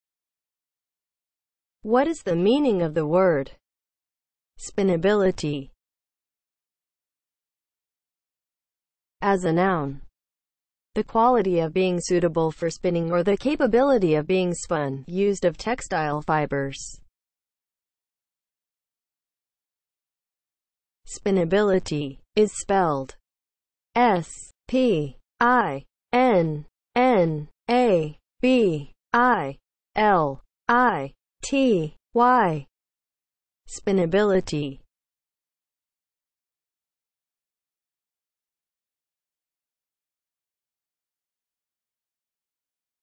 What is the meaning of the word SPINNABILITY?
Definition and spelling of the word SPINNABILITY.

(00:00) Intro
(00:08) As a noun
(00:19) Spelling

As a noun (00:08):
• the quality of being suitable for spinning or the capability of being spun (used of textile
  fibers)